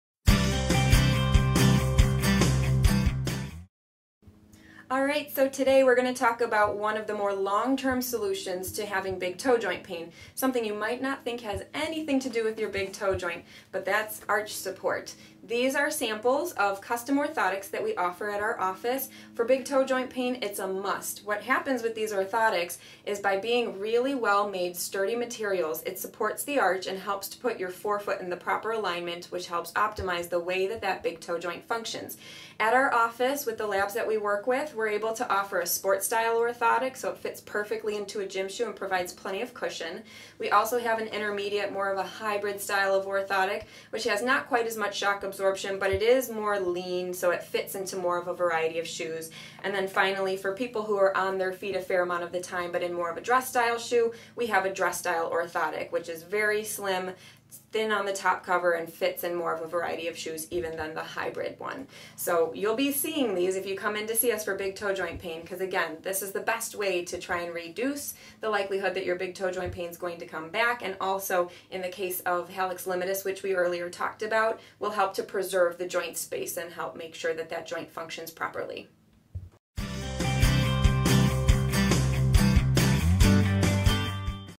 BigToeJointPain Installment 6: Custom Orthotics!
BigToeJointPain installment 6: Custom Orthotics! Dr. Doyle explains the options and how they can help!